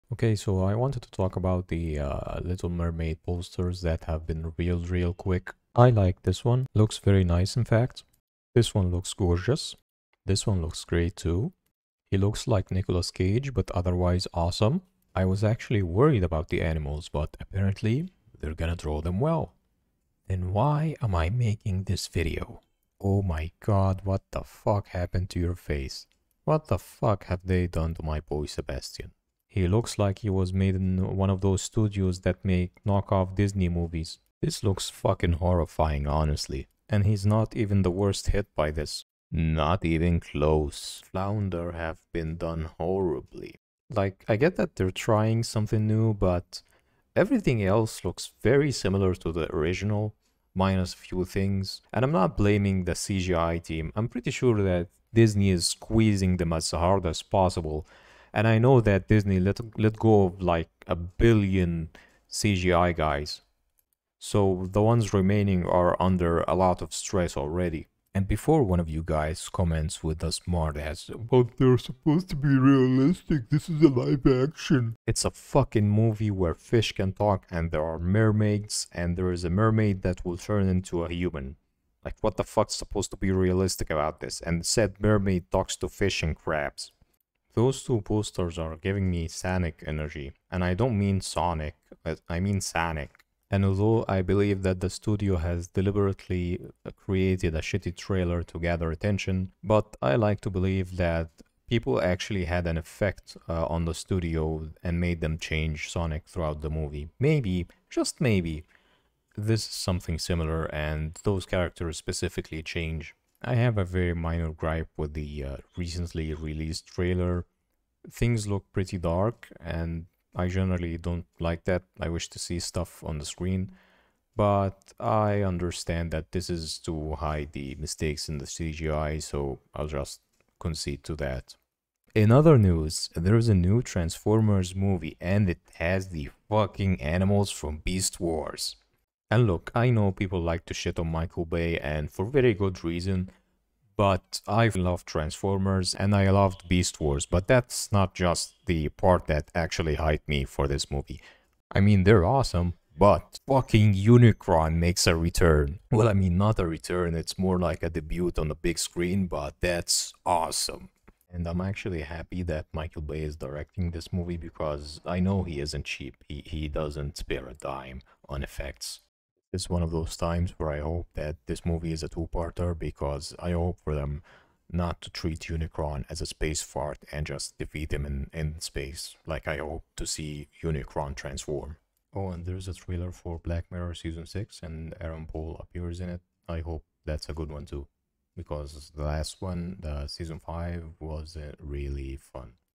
The Little Mermaid Reveal: WHY? Also TRANSFORMERS BEAST WARS
This might be a deviation from my normal content, if you hate them i'll not make more of those
I saw the reveal for the little mermaid, the animals really turned me off.
Transformers trailers looks amazing though, really hyped for it.
For those of you who will comment saying this stuff is for kids you suck.